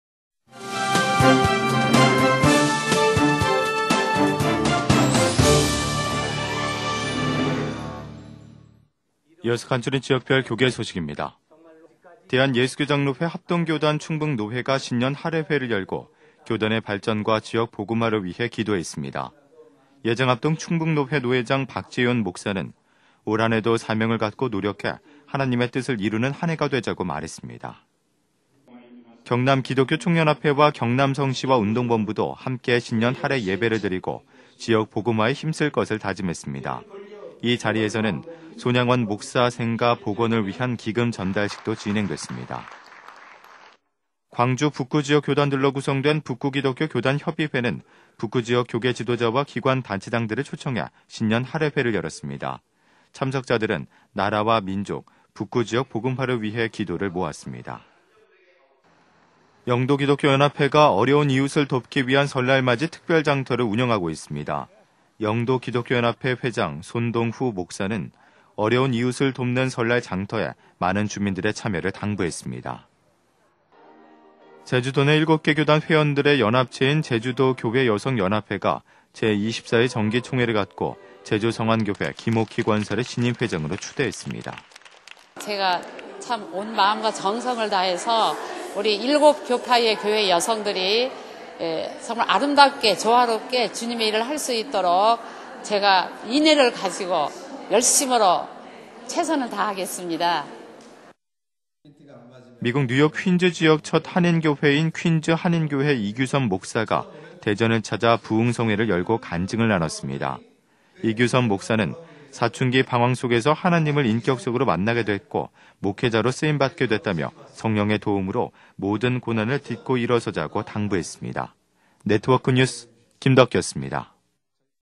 [네트워크] 충북,경남,광주,부산,제주,대전
[충북]예장합동 충북노회 신년하례회 / 청주 중앙교회

대한예수교장로회 합동교단 충북노회가 신년하례회를 열고 교단의 발전과 지역복음화를 위해 기도했습니다.

예장합동 충북노회 노회장 박재윤 목사는 "올 한해도 사명을 갖고 노력해하나님의 뜻을 이루는 한 해가 되자"고 말했습니다.

[경남]경남기총, 성시화운동본부 공동 신년하례예배 / 창원 인터내셔널 호텔

경남기독교총연합회와 경남성시화운동본부도 함께 신년하례예배를 드리고 지역복음화에 힘쓸 것을 다짐했습니다.

이자리에서는 손양원목사 생가 복원을 위한 기금 전달식도 진행됐습니다.

[광주]광주 북구기독교교단협의회 신년하례회 / 광주 동암교회

광주 북구지역 교단들로 구성된 북구 기독교 교단협의회는 북구지역 교계지도자와 기관단체장을 초청해
신년하례회를 열었습니다.

참석자들은 나라와 민족,북구지역 복음화를 위한 기도를 모았습니다..

[부산]네트웤크/설날맞이 특별장터 개최 / 부산 제2영도교회

영도기독교연합회는가 어려운 이웃을 돕기 위한 설날맞이 특별장터를 운영하고 있습니다.

영도기독교연합회 회장 손동후 목사는어려운 이웃 돕는 설날 장터에 많은 주민들의 참여를 당부했습니다.

[제주]제주도교회여성연합회 정기총회/제주 성안교회

제주도 내 7개 교단 회원들의 연합체인 제주도교회여성연합회가 제24회 정기총회를 갖고 제주성안교회 김옥희 권사를 신임회장으로 추대했습니다.

제주도교회여성연합회 신임회장 제주성안교회 김옥희 권사

[대전]대전열방교회 신년축복 부흥성회 / 대전 대덕구 읍내동

미국 뉴욕 퀸즈 지역 첫 한인교회인 퀸즈한인교회 이규섭 목사가 대전을 찾아 부흥성회를 열고 간증을 나눴습니다.

이규섭 목사는 "사춘기 방황 속에서 하나님을 인격적으로 만나게 됐고 목회자로 쓰임받게 됐다"며, "성령의 도움으로모든 고난을 딛고 일어서자"고 당부했습니다.